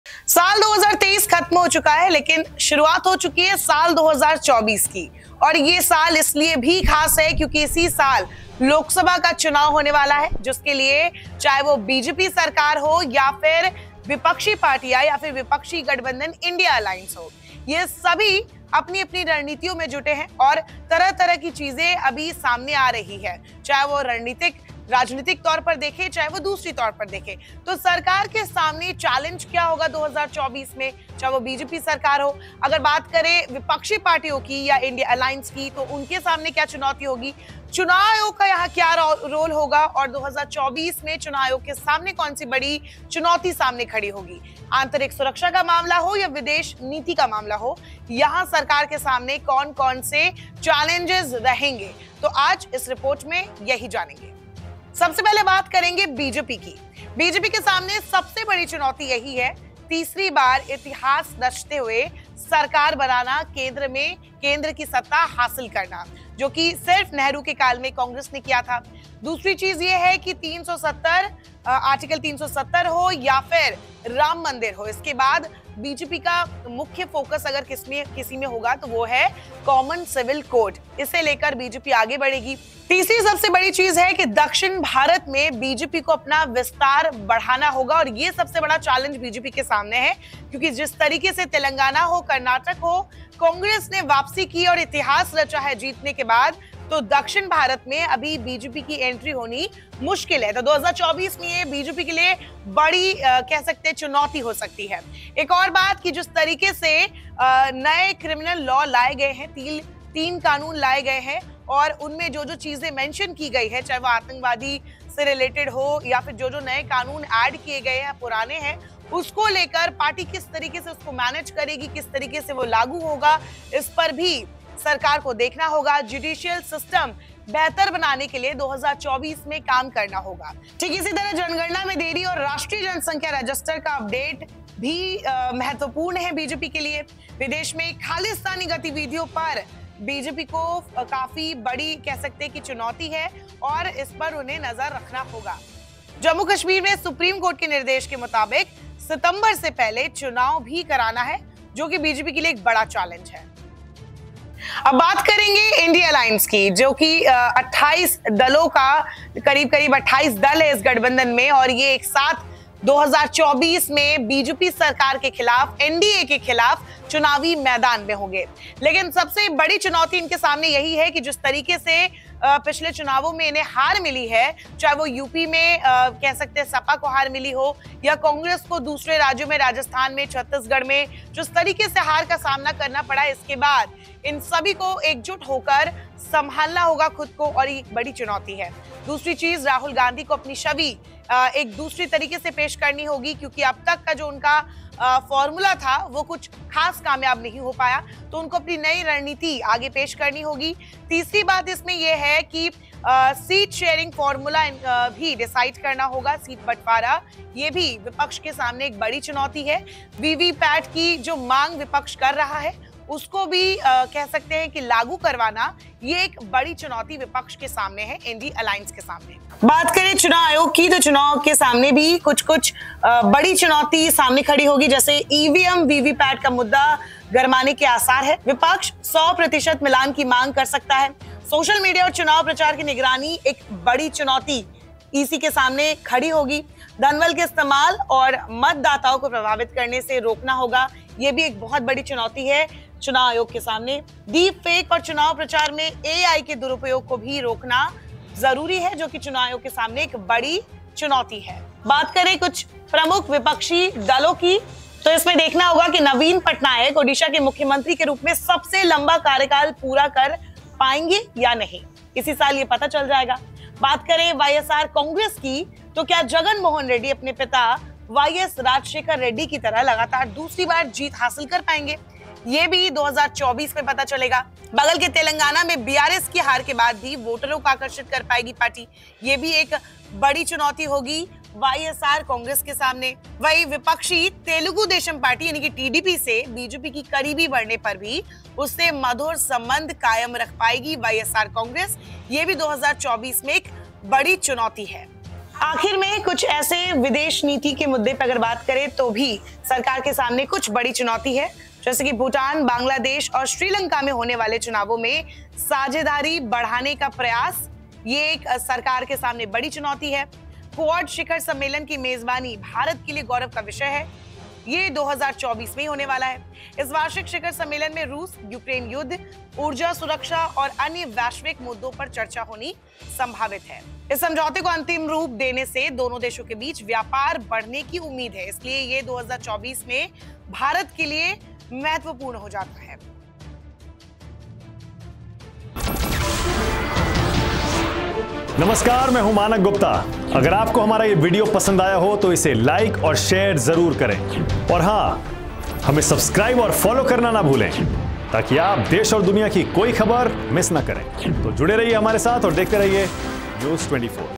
2024 में BJP लगाएगी तीसरी बार हैटट्रिक I.N.D.I.A दलों की चुनौती क्या?, 2024 की सियासी भविष्यवाणी
2024 में BJP को मिलेगी तीसरी बार सत्ता या I.N.D.I.A करेगी कुछ कमाल, जानें इस साल सरकार के सामने क्या है बड़ा चैलेंज?